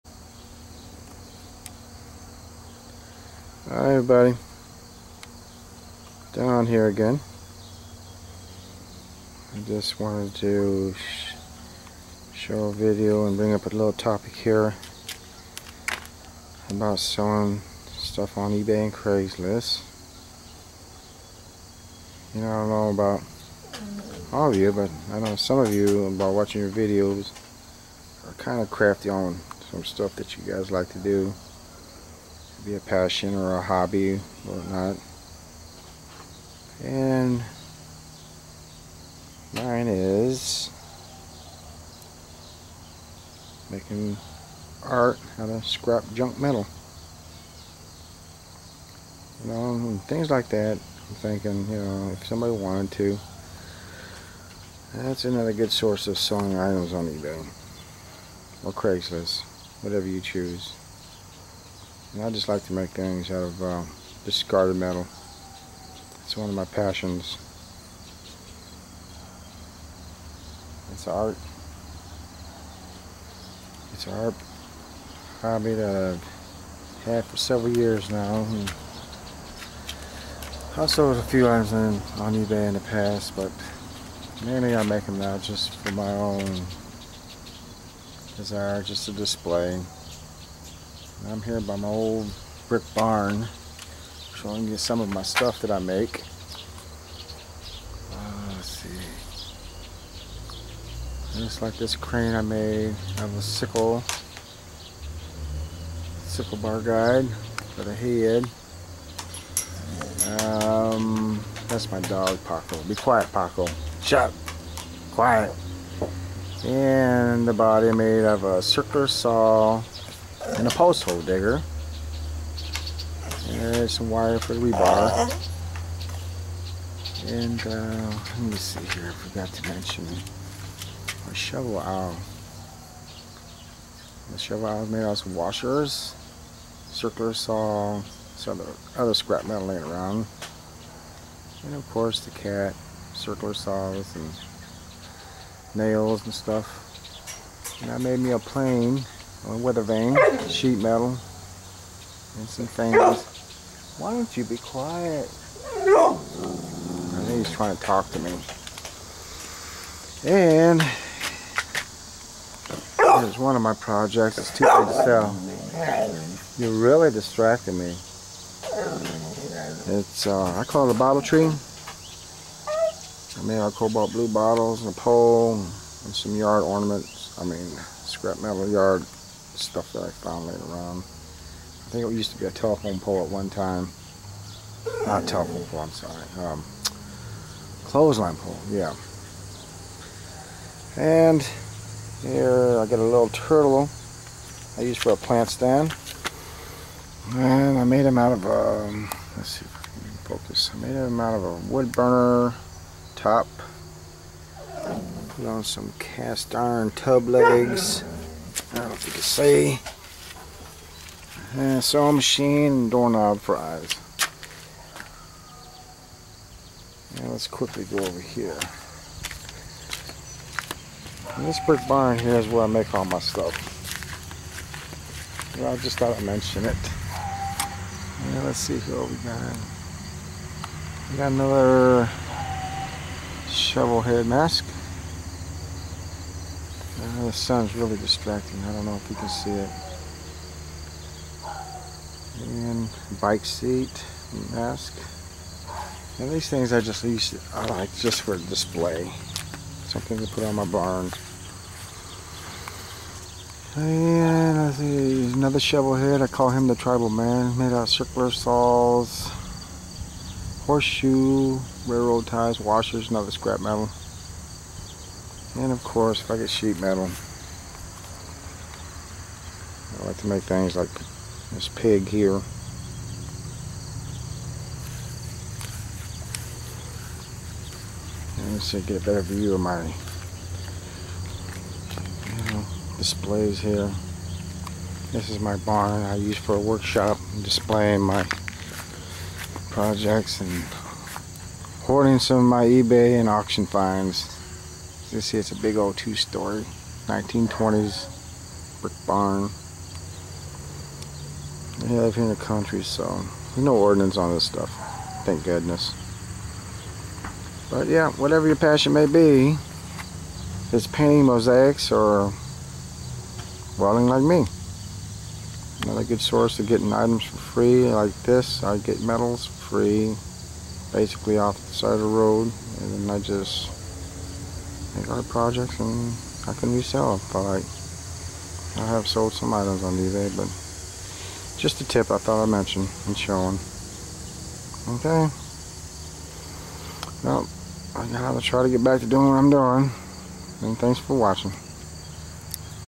Making Money / Ebay / Craigs List Selling Art / Junk Metal Yard Sculptures Sodus, MI
Here is a sample of what I like to make when time and material is available.   I call it Junk Metal Yard Art.  Thought to show the idea of those who are crafty on what they like to make.  You could sell the items you make and sell on Ebay or Craigs list as well and profit from what you like to produce.  The items I make are metal sculptures from discarded junk metals I find or get real cheap at yard sales.   I have sold some items in the past for descent prices.   Just thought to share if you have a crafty idea or into making art projects. Thanks for looking!.